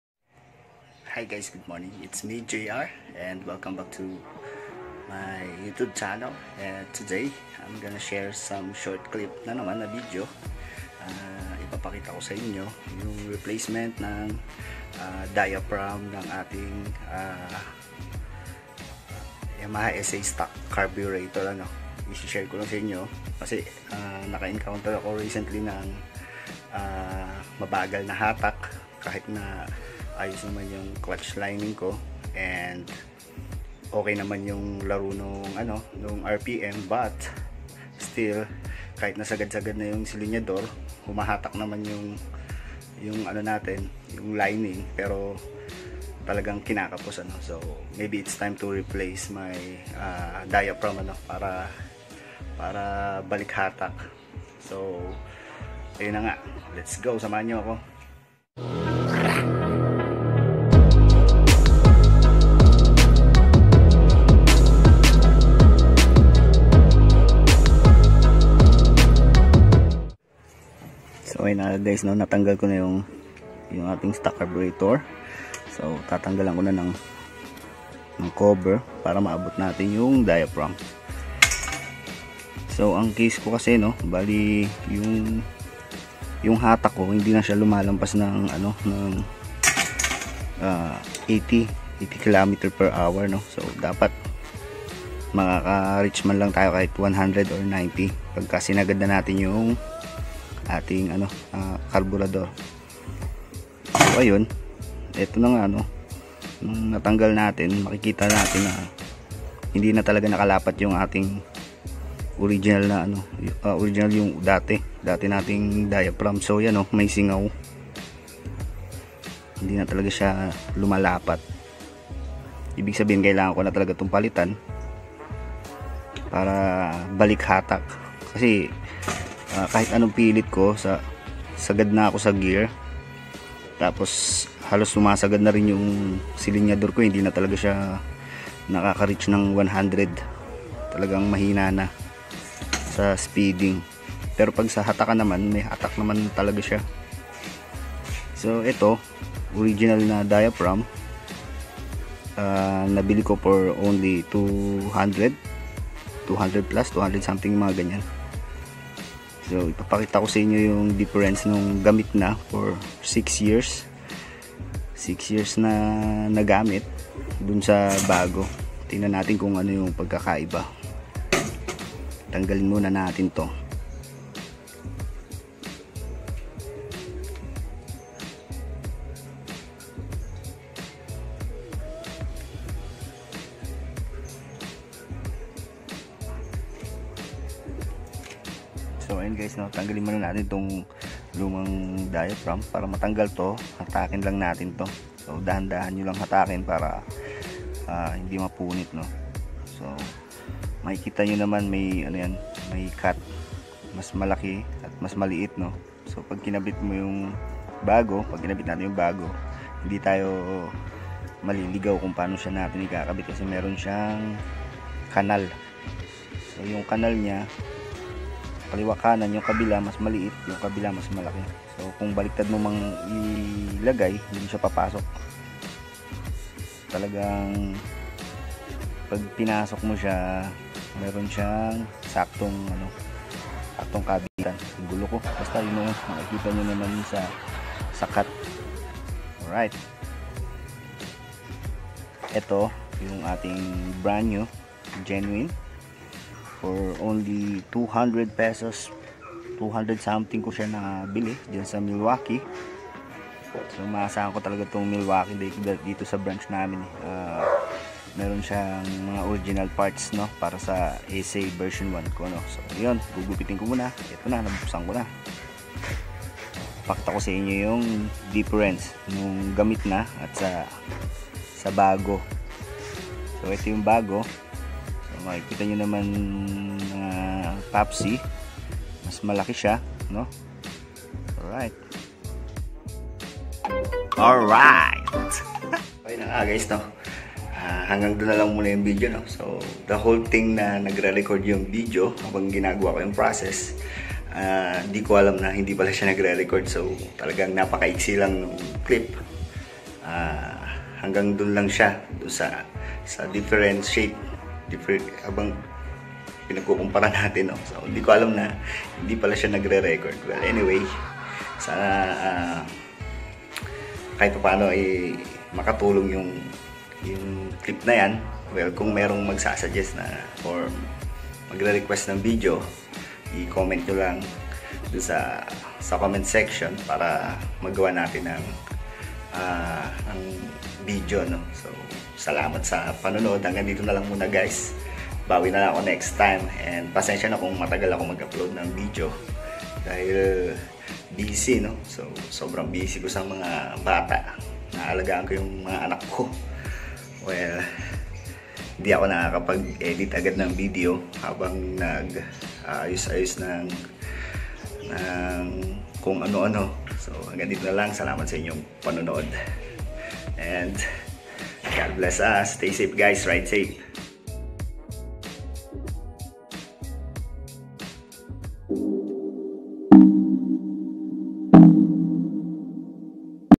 YAMAHA SZ16 | DIAPHRAGM REPLACEMENT | STOCK CARBURETOR
Hi guy's good day, sharing another Short clip, regarding on my yamaha sz16 stock carburetor, replacing the worn-out diaphragm, thanks stay safe Ridesafe God bless us.